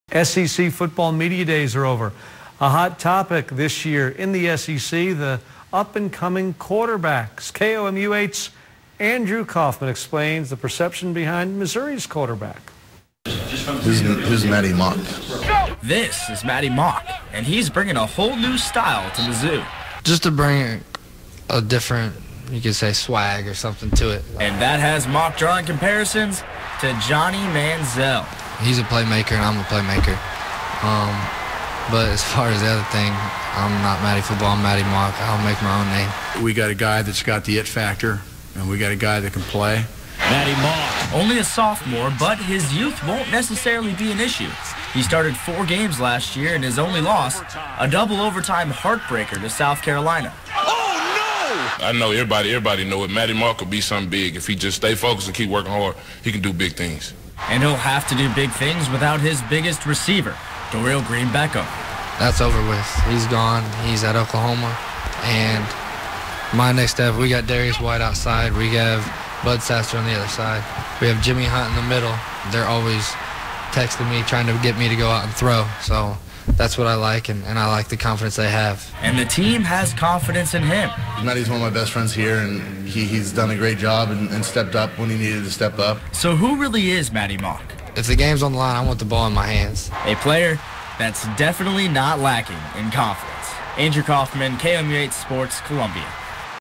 Who is Maty Mauk?
KOMU 8's Andrew Kauffman explains the perception behind Missouri Quarterback Maty Mauk.